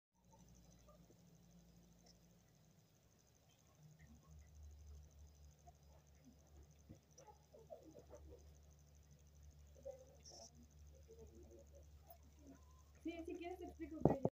20 de febrero de 2024
Blood sample, obtained by centrifugation, which in the upper part has the serum with the appropriate color (straw) for a diagnosis and in the lower part the erythrocytes are found in red.
Muestra de sangre, obtenida por centrifugación, que en la oarte superior tiene el suero con el color adecuado(pajiso) para un diagnóstico y en la parte baja se encuentran los eritrocitos en color rojo.